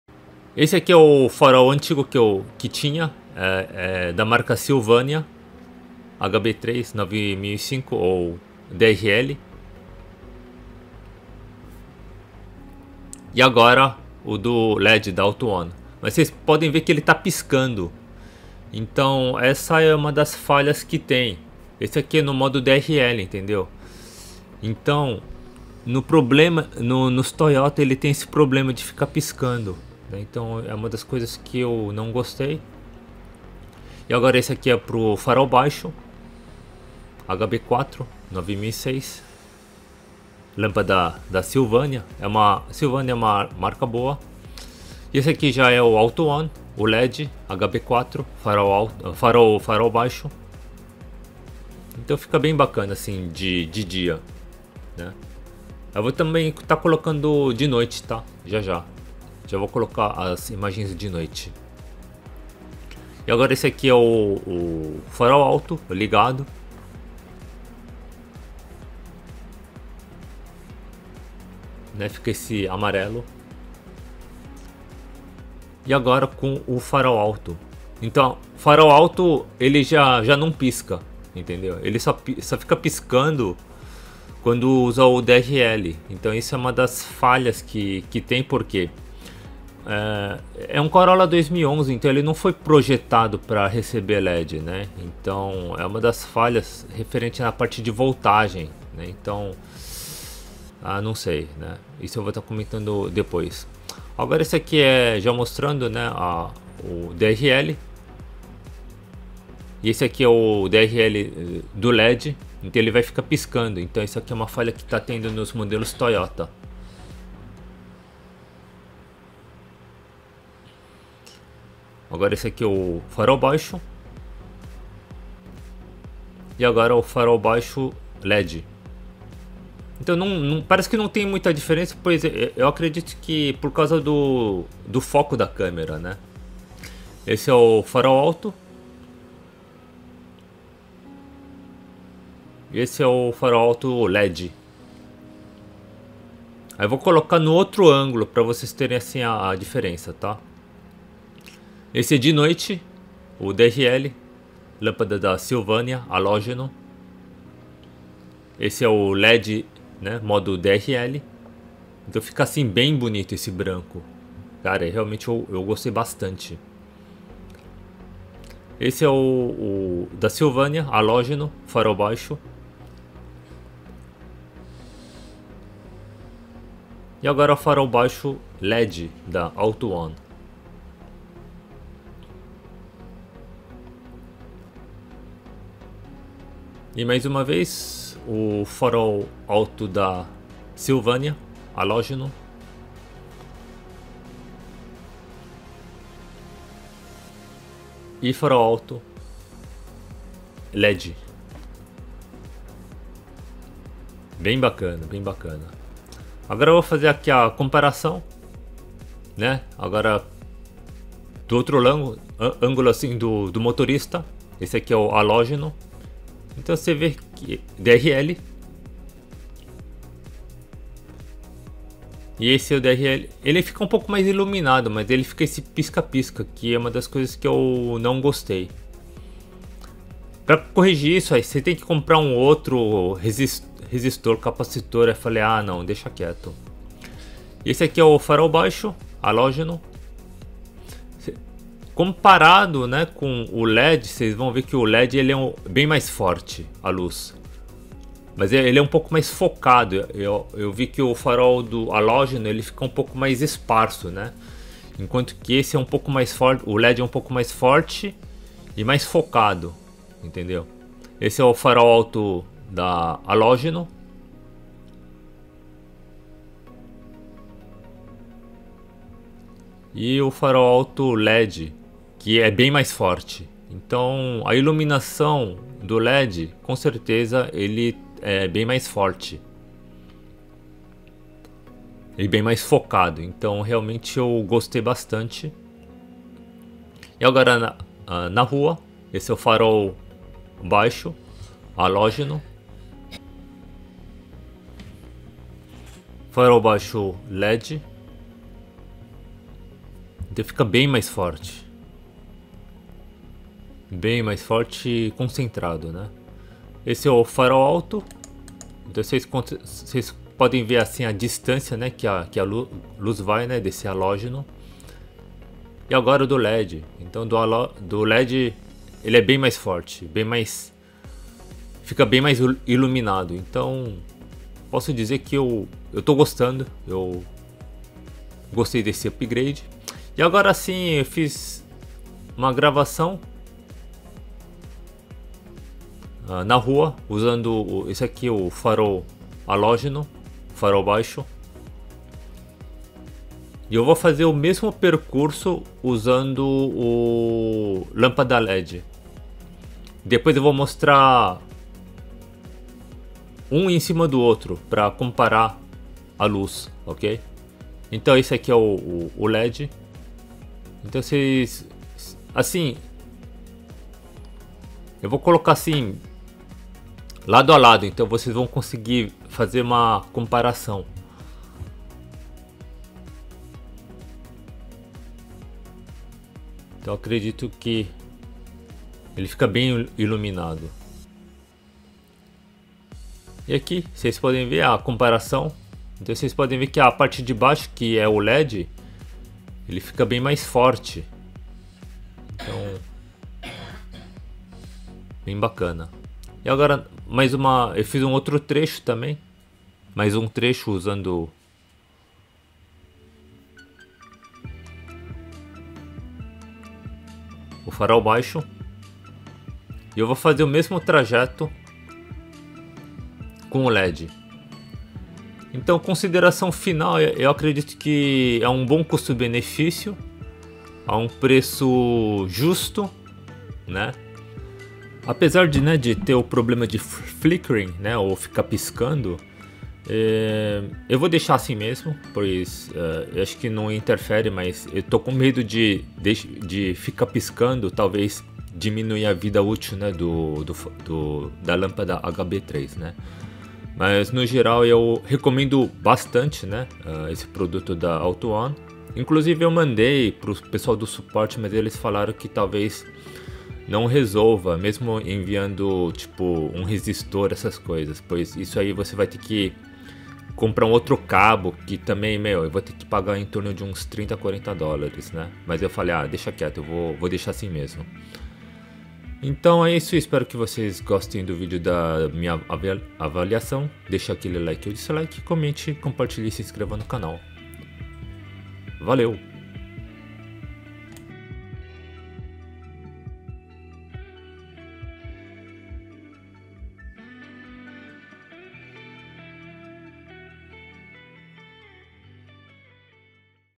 Lâmpada Halógena (Sylvania) vs LED (Autoone) - Toyota Corolla 2011
Lâmpada automotiva - Comparação Halógena (Sylvania) vs LED (Autoone)

- Amazon HB3 / 9005

- Amazon HB4 / 9006